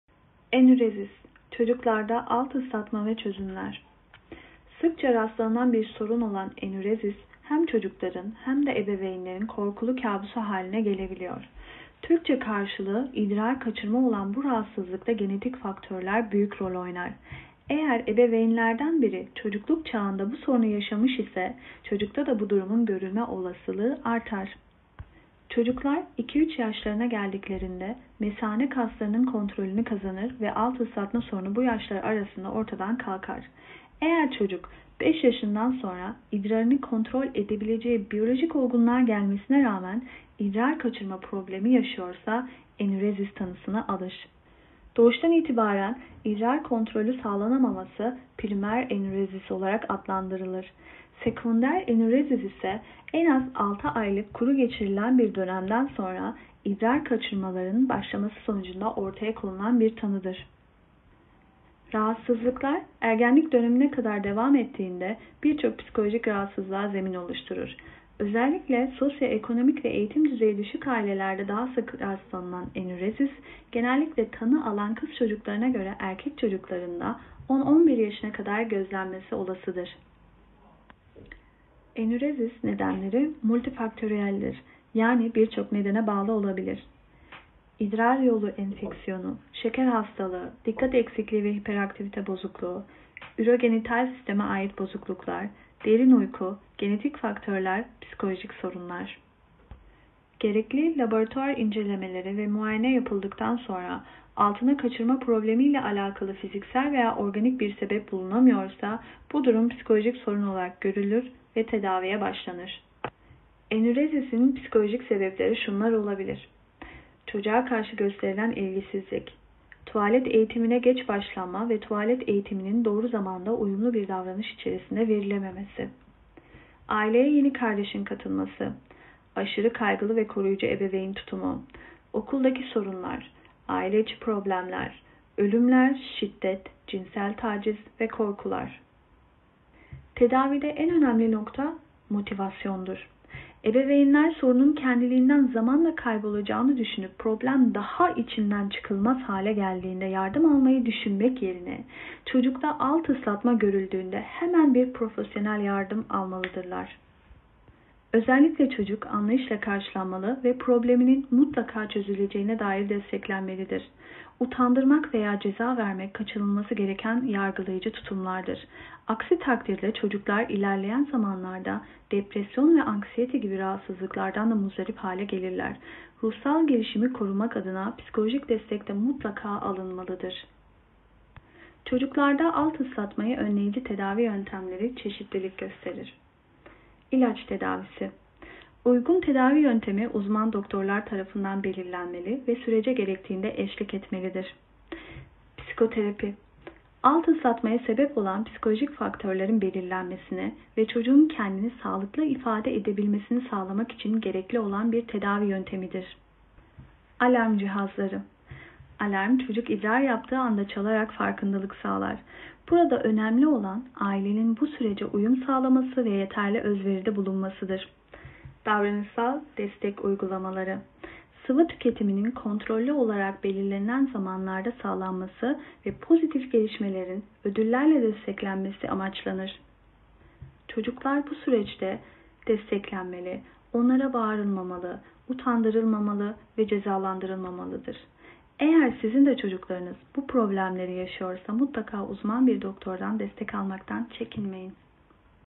ENÜREZİS- Melike İnce (Sesli Makale)
Sinir ve Psikoterapi Bilimi'nde çalışmalar yapan Melike İnce'nin kendi sesinden güncel makalesi ENÜREZİS - Çocuklarda Alt Islatma ve Çözümleri dinlediğiniz için teşekkür ederiz.